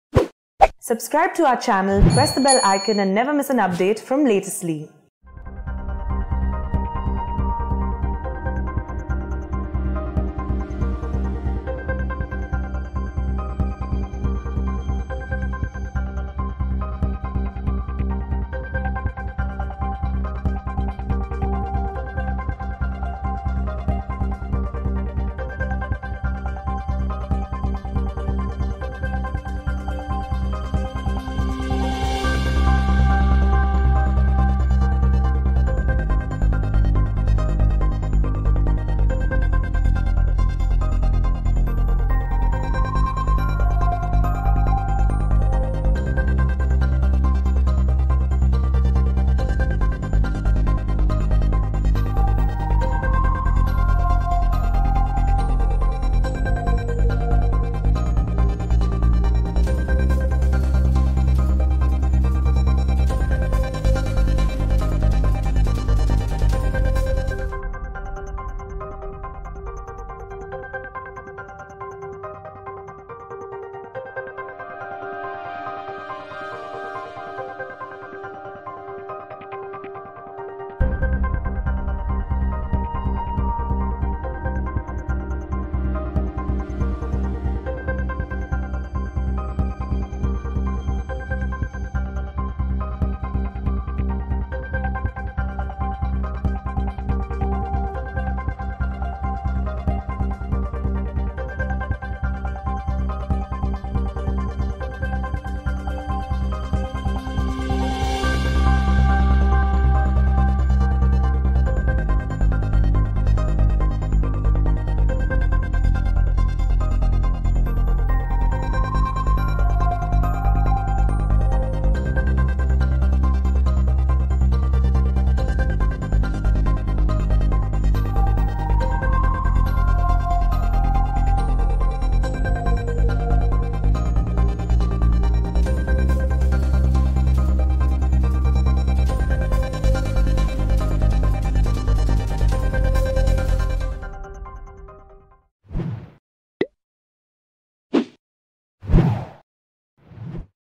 Srinagar: अमीरा कादल मार्केटमध्ये दहशतवादी हल्ला, घटनेत एकाचा मृत्यू, २३ जण जखमी
दुपारी 4:20 वाजता, दहशतवाद्यांनी हरिसिंह हाय स्ट्रीटवर तैनात पोलीस आणि सुरक्षा दलांवर ग्रेनेड फेकले”असे पोलीस अधिकाऱ्याने सांगितले. हा भीषण हल्ला सुरक्षा कॅमेऱ्यात कैद झाला आहे.